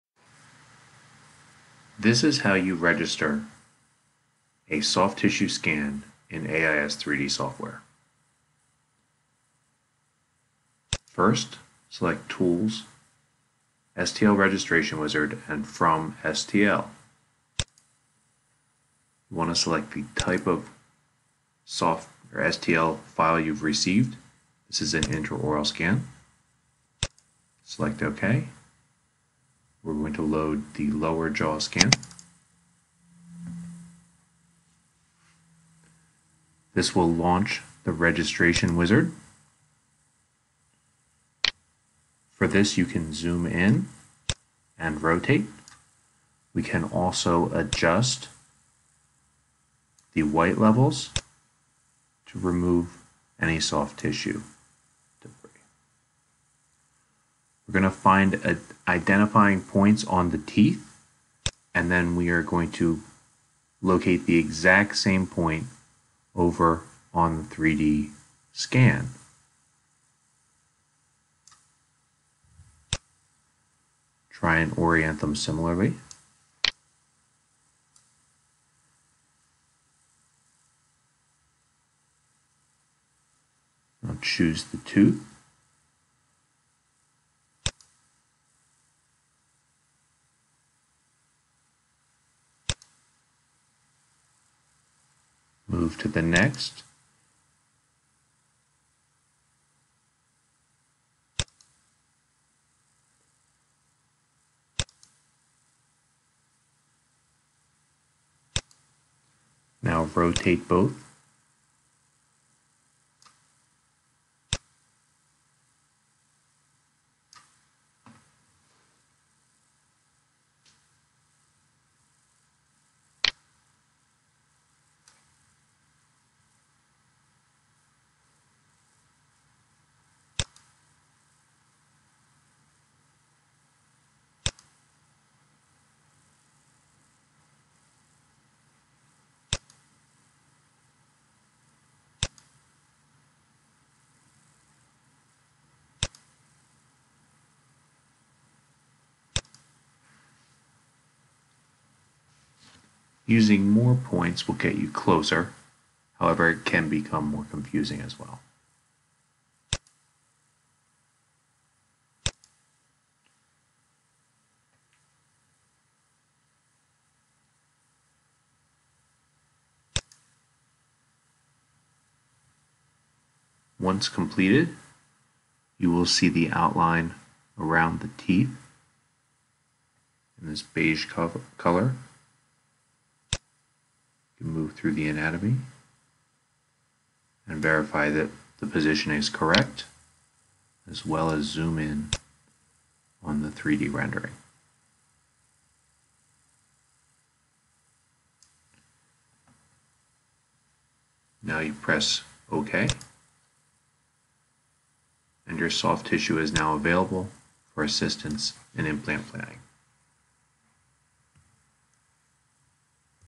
STL Overlay in AIS
Step by step instructions on how to register a soft tissue scan in ACTEOn Imaging Suite  (AIS) software with the X-Mind Trium